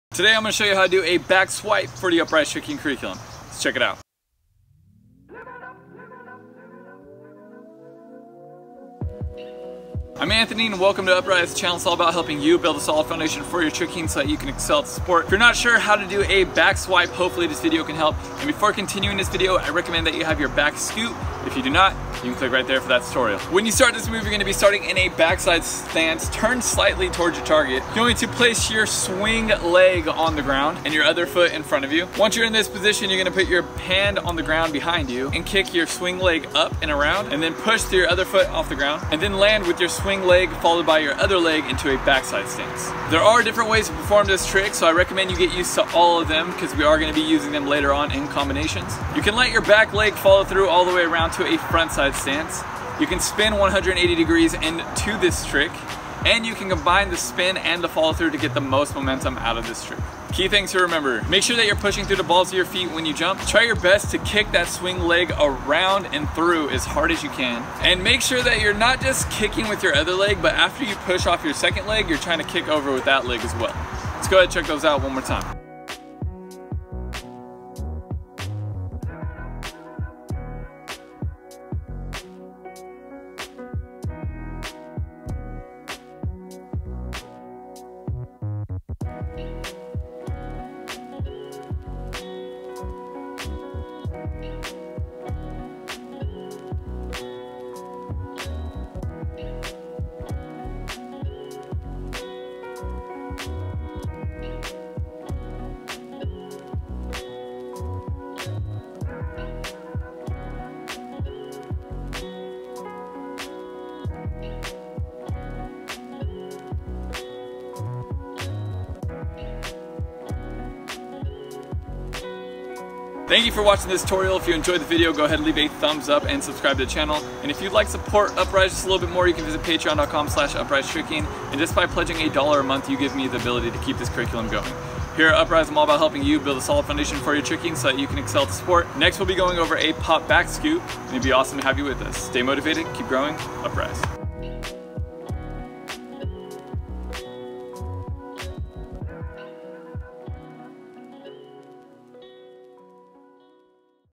How to Back Swipe | Tricking Tutorial #72
Learn how to do a Back Swipe for the Upraiz Tricking curriculum! A step-by-step tutorial for Trickers that want to build their foundation and excel at Tricking!

Stay motivated, keep growing, Upraiz.